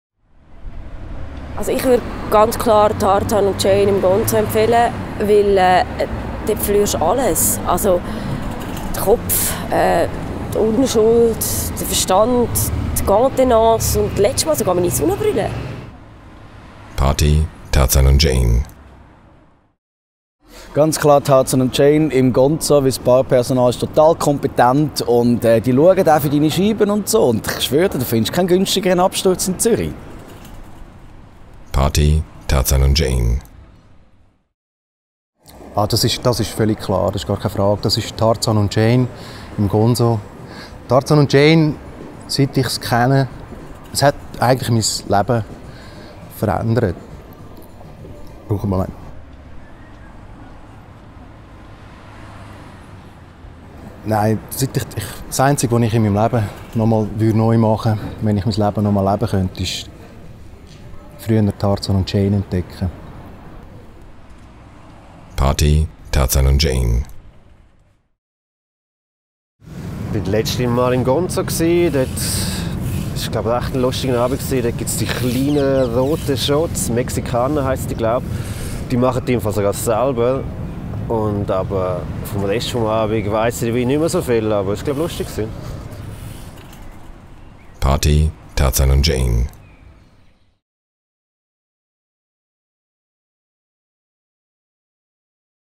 PARTY: TARZAN&JANE. (3/3) - PARODY #5
This is an unauthorized commercial!

TARZAN & JANE
FRIDAY 27. MÄRZ 2015, 23H

TEAM:

Schauspieler: Rosanna Grüter, Nici Faerber, Rafi Hazera, Olivier Mächler
Sprecher: Heiner Butz